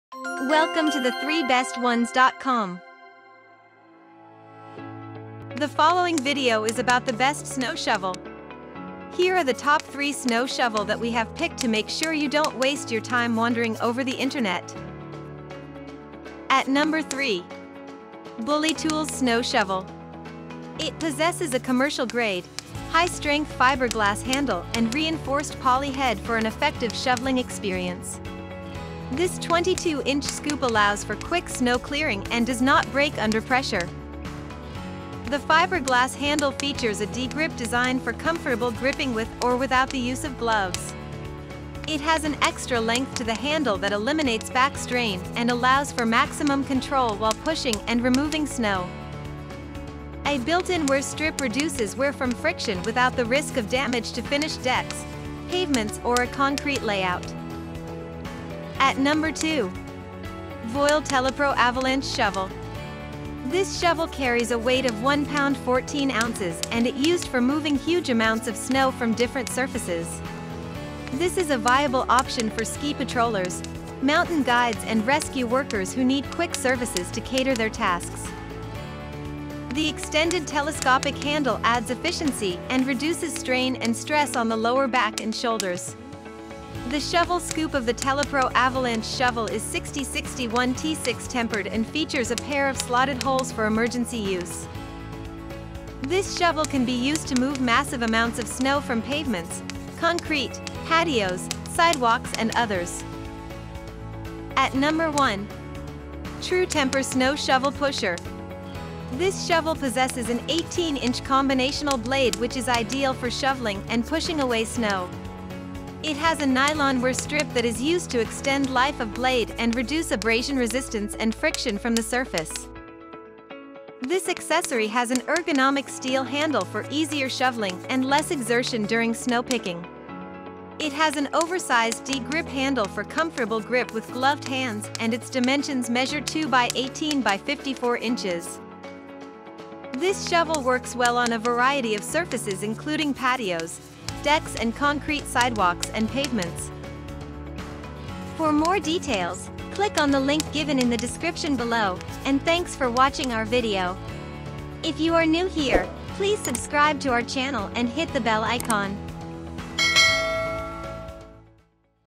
Find The Best Snow Shovels In Just 2 Minutes
Hurry up! Don't miss out on this chance to get rewarded! Enter now for your shot at exciting prizes!

Follow the links given below to check the product prices:

Here is the list of the top 3 best "Snow Shovels":

True Temper Snow Shovel / Pusher:

Voile Telepro Avalanche Shovel:

Bully Tools 92814 Snow Shovel:

0:00​​ Best Snow Shovels Reviewed
0:16 Bully Tools 92814 Snow Shovel
0:58 Voile Telepro Avalanche Shovel
01:44 True Temper Snow Shovel / Pusher
02:28​​ Closing Remarks & Subscribe To Our Channel